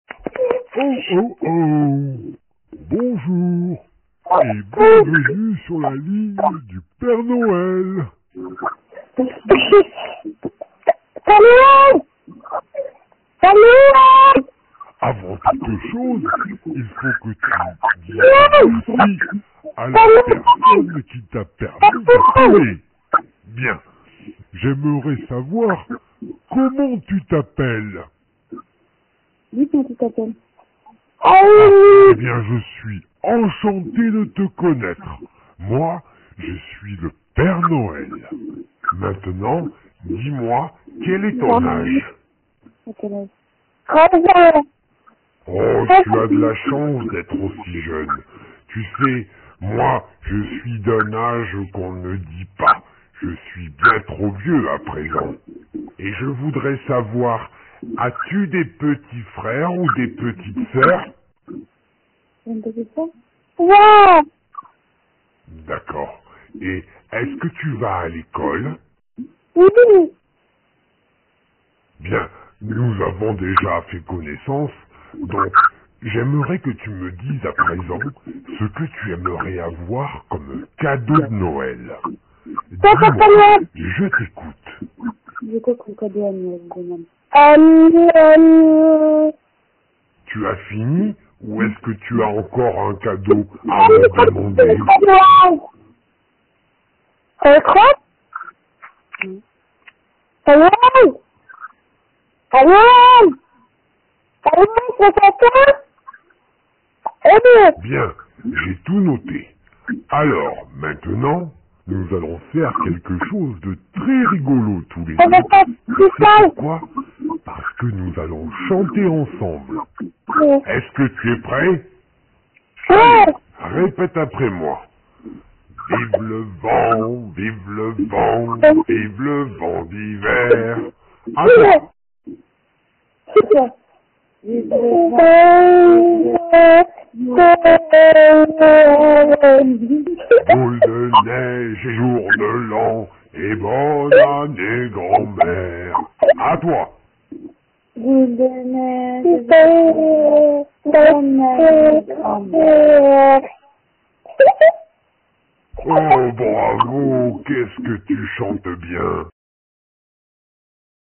Maena a appelé le numéro du Père Noël et discute avec lui le 13-12-2014 a 10:55:38
Ecoutez Maena discuter avec le Père Noël, chanter avec lui et lui donner sa liste de cadeaux de Noël.